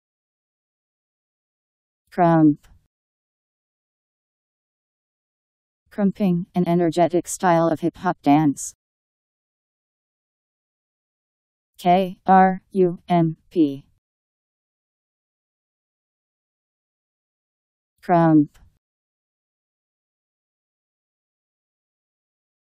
Krump Meaning
Video shows what krump means. Krumping: an energetic style of hip-hop dance.  Krump Meaning. How to pronounce, definition audio dictionary. How to say krump. Powered by MaryTTS, Wiktionary